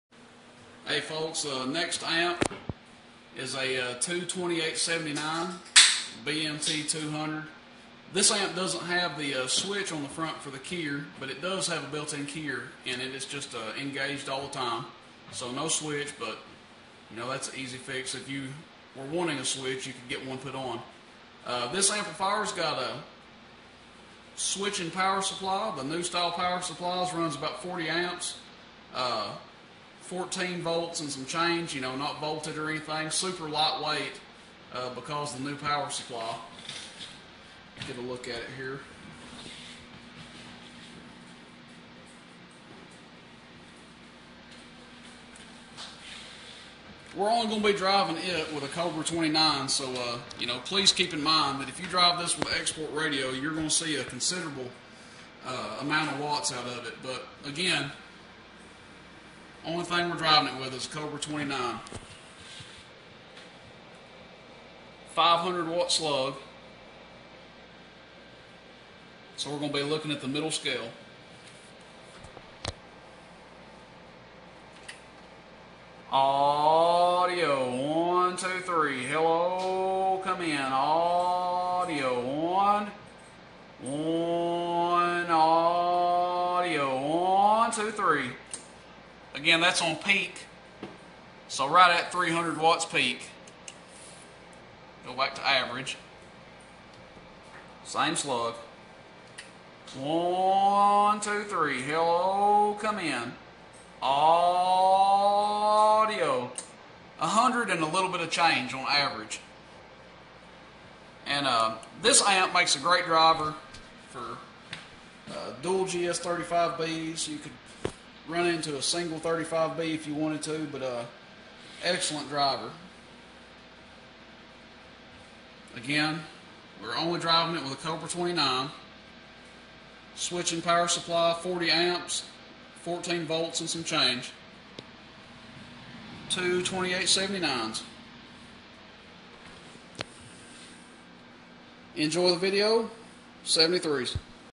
BMT200 Two 2879(regulated power supply)
***Has hot and ground hookup on the back for you to power your mobile radio***
*40 amp regulated power supply
* Built in keyer (cancels out the need for a foot pedal on your big box)
*Runs very cool and efficient for looooong keydowns.